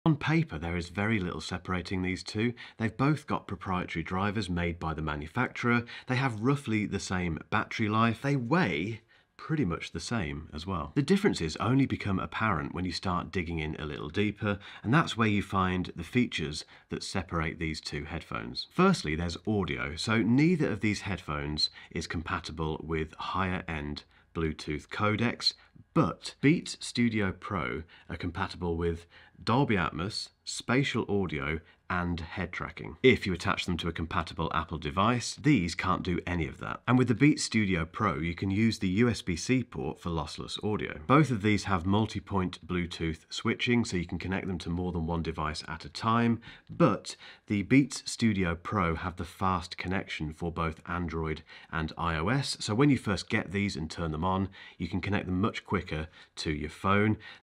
Beats Studio Pro vs Bose QC45
It’s time for the battle of the Beats Studio Pro vs the Bose QC45. Which are the best daily drivers?

GET A PEEK BEHIND THE SCENES:
📧 Sign up to my newsletter and receive a behind-the-scenes video from me every week